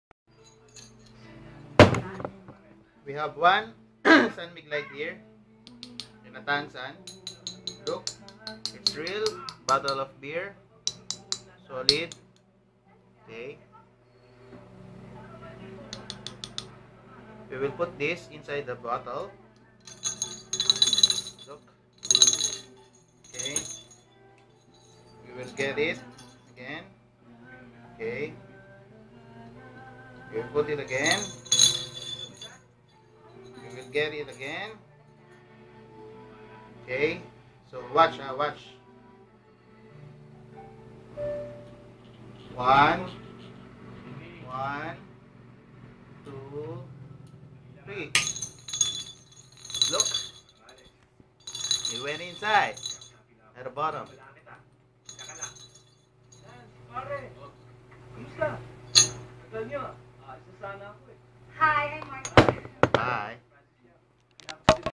Cap thru Glass Beer Bottle
Metal cap of Glass Beer bottle penetrating thru the bottom of the bottle . . . . please rate my video thanks!